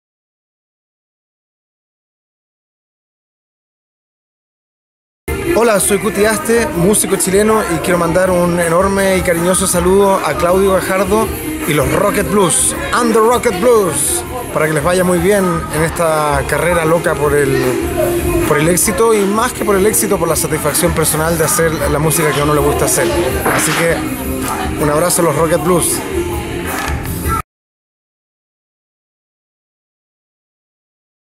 (8). Cuti Aste SALUDA a Claudio Gajardo
Cuti Aste saluda a Claudio Gajardo.
Lugar: Centro Cultural Espacio Matta (La Granja, Región Metropolitana, Chile)